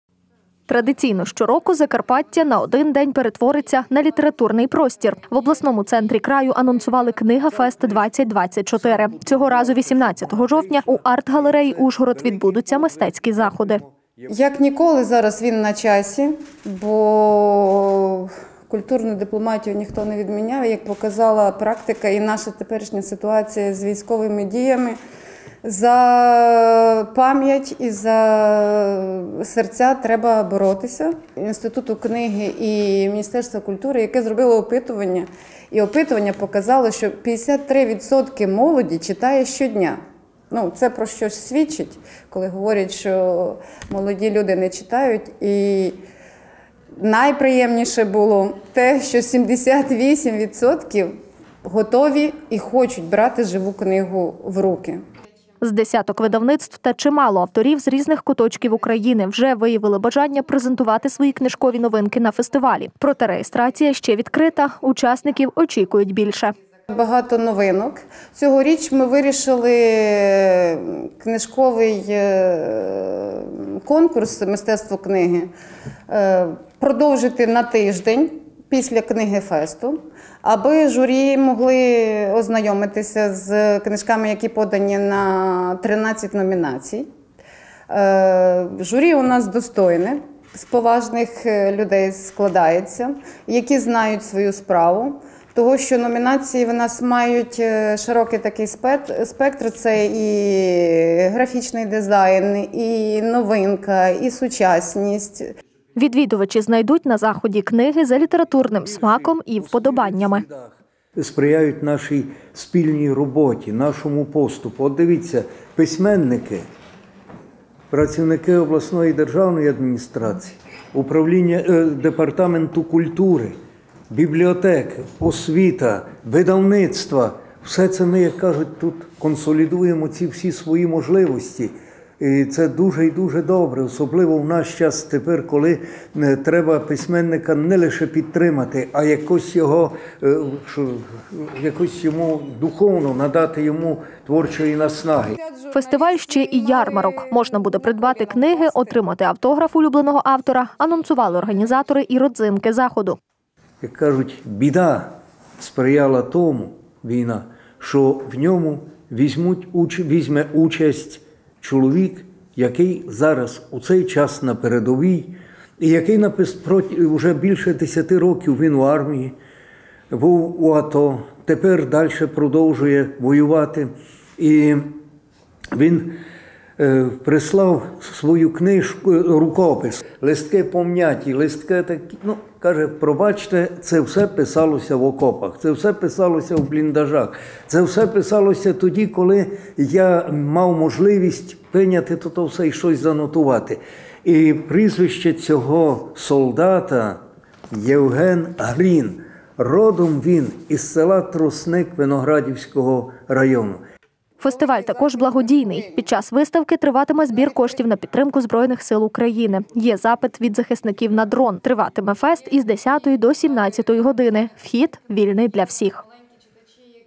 «Книга-Фест-2024»
Традиційно щороку Закарпаття на один день перетвориться на літературний простір. В обласному центрі краю анонсували «Книга-фест -2024». Цього разу 18 жовтня у арт - галереї  «Ужгород» відбудуться мистецькі заходи.  З десяток видавництв та чимало авторів з різних куточків України вже виявили бажання презентувати свої книжкові новинки на фестивалі. Проте реєстрація ще відкрита, учасників очікують більше.   Відвідувачі знайдуть на заході книги  за літературним смаком і вподобаннями.  Фестиваль -  ще й ярмарок. Можна буде придбати  книжки, отримати автограф улюбленого автора. Анонсували організатори і родзинки заходу. Захід також благодійний. Під час виставки триватиме збір коштів на підтримку ЗСУ – є запит від захисників на дрон. Триватиме фест з 10 до 17 години. Вхід вільний.